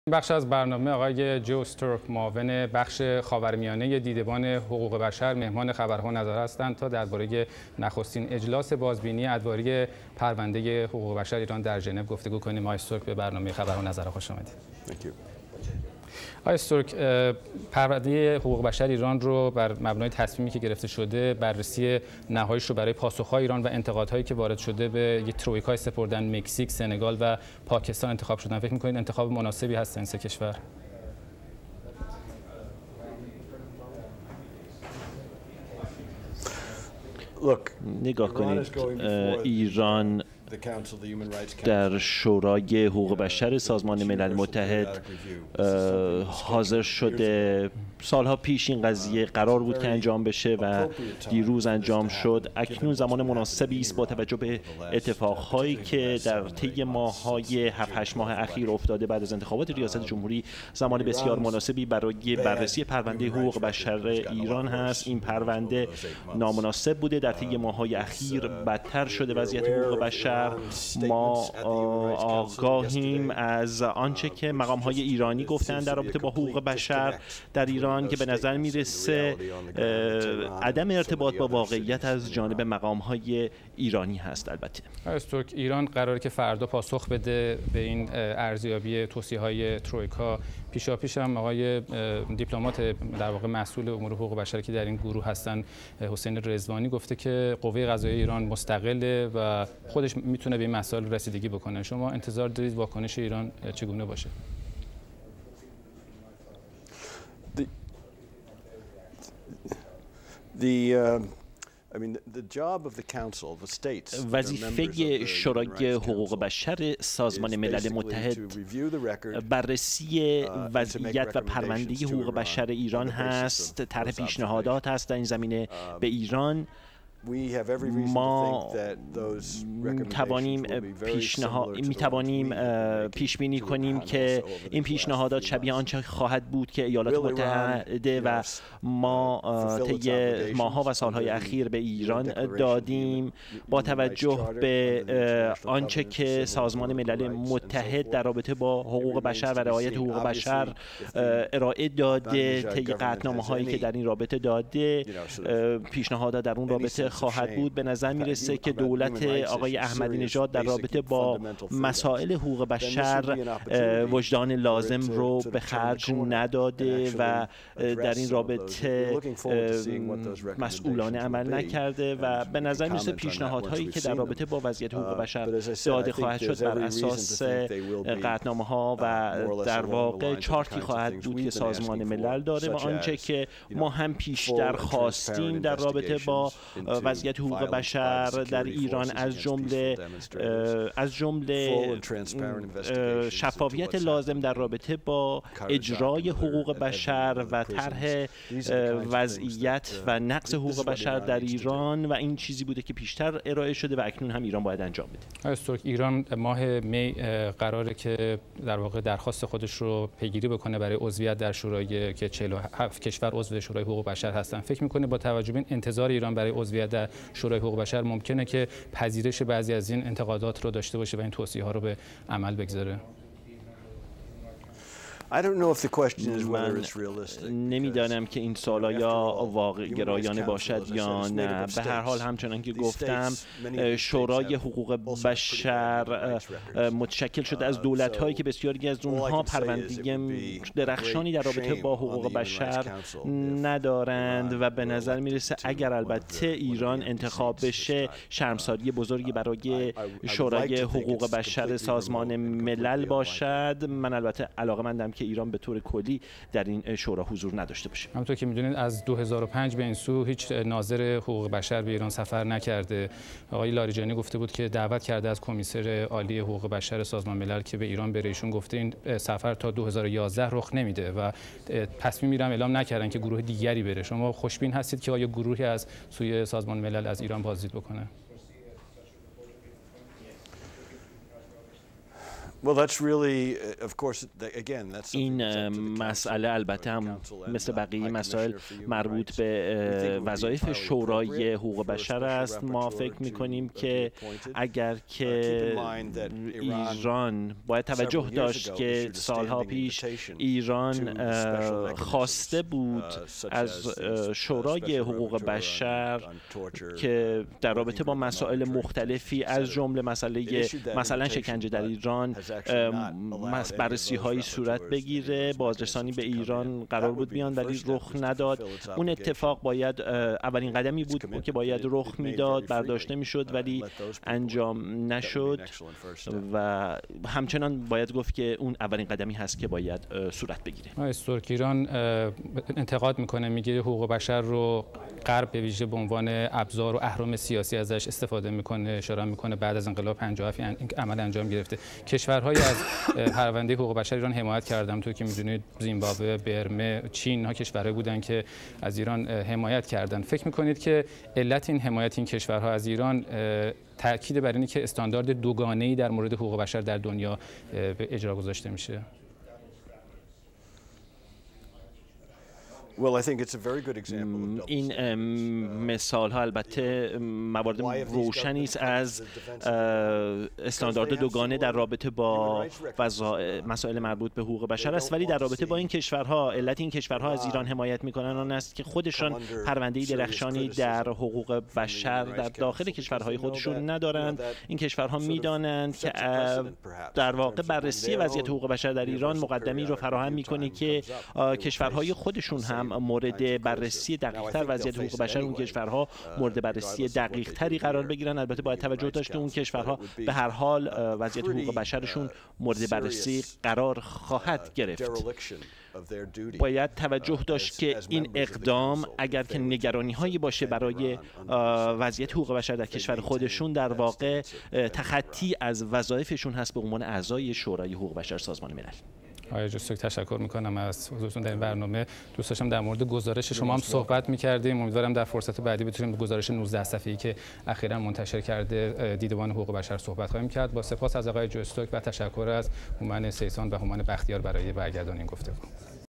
Joe Stork in VOA
Siamak Dehghanpour's Interview with Joe Stork, deputy director of Human Rights Watch's Middle East and North Africa division